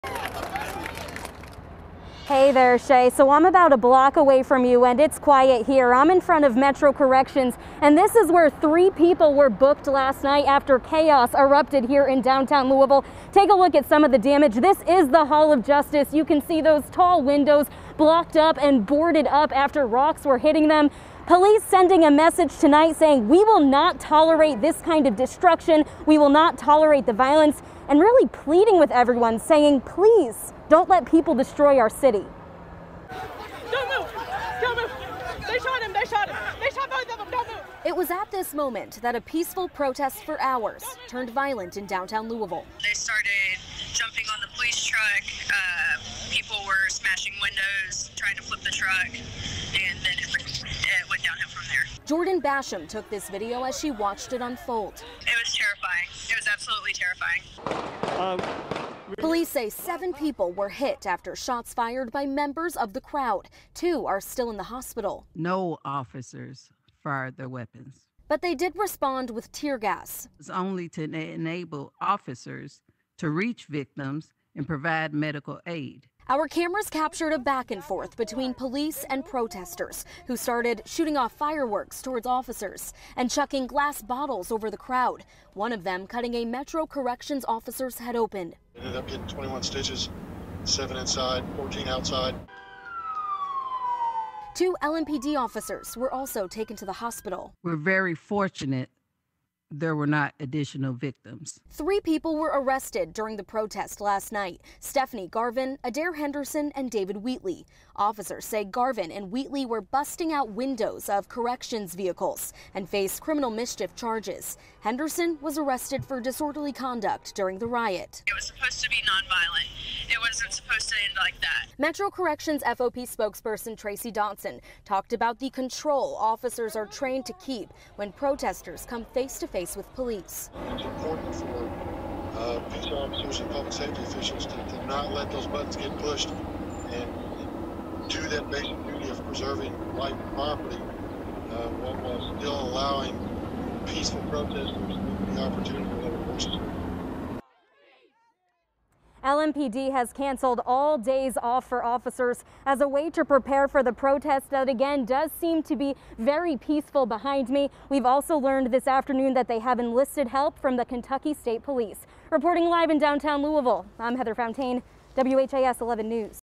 Arrests made in Louisville's Breonna Taylor protest
Arrests were made, though none were connected to the shooting that injured seven Thursday night.